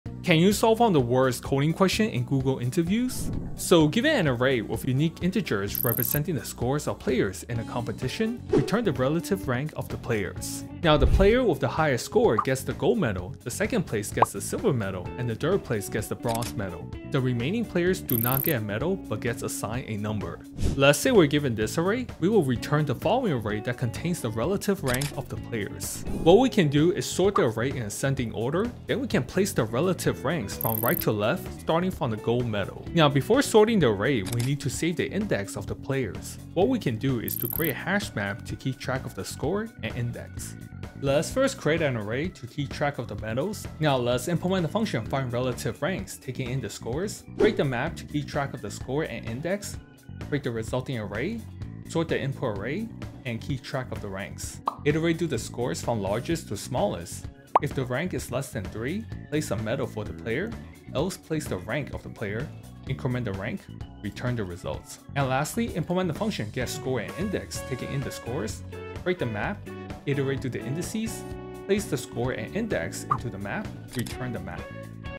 One of the worst coding question in Google Interviews
LeetCode 506. Relative Ranks

You are given an integer array score of size n, where score[i] is the score of the ith athlete in a competition. All the scores are guaranteed to be unique.

The athletes are placed based on their scores, where the 1st place athlete has the highest score, the 2nd place athlete has the 2nd highest score, and so on. The placement of each athlete determines their rank:

The 1st place athlete's rank is "Gold Medal".

The 2nd place athlete's rank is "Silver Medal".

The 3rd place athlete's rank is "Bronze Medal".

For the 4th place to the nth place athlete, their rank is their placement number (i.e., the xth place athlete's rank is "x").

Return an array answer of size n where answer[i] is the rank of the ith athlete.

Time Complexity = O(nlogn), where 'n' is the length of the input array

Space Complexity = O(n), where 'n' is the length of the input array